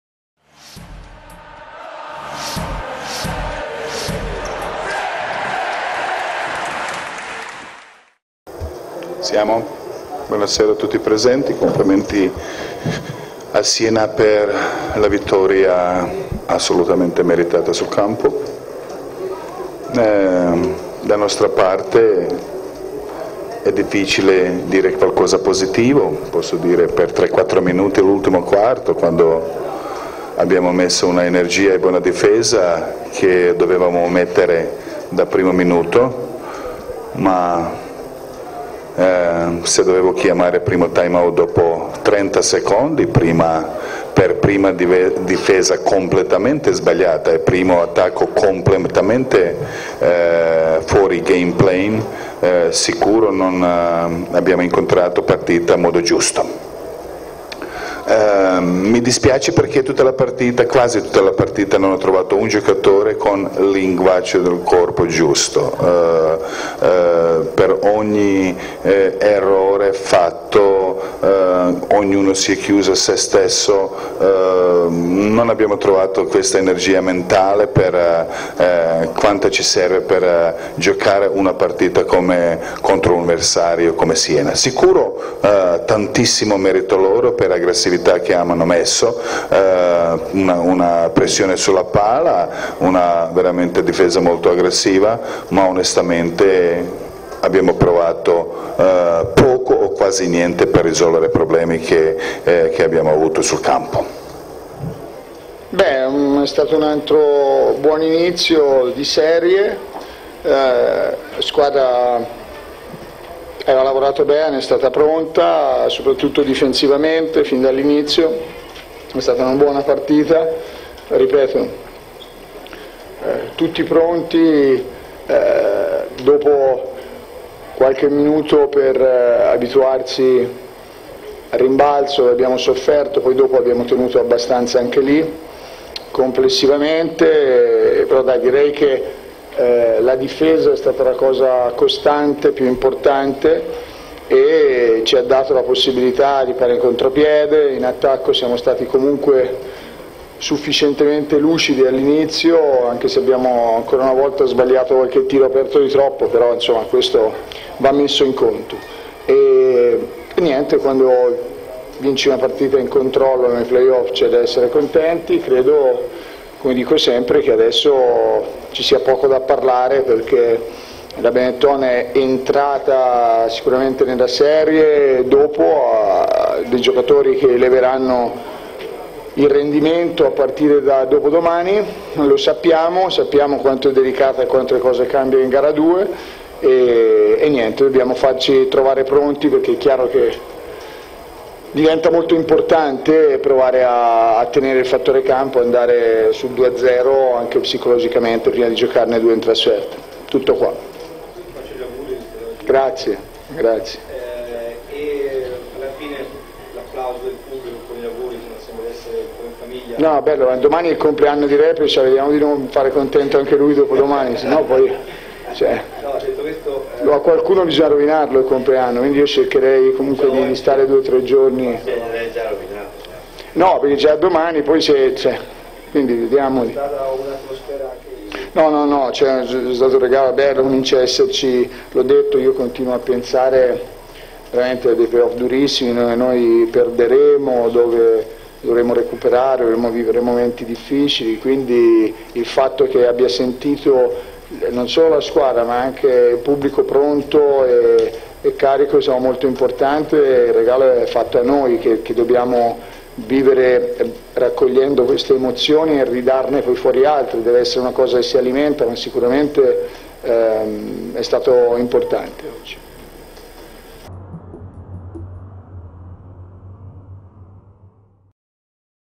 Montepaschi-Benetton (G-1) La Sala Stampa
Jasmin Repesa e Simone Pianigiani commentano gara-1 delle semifinali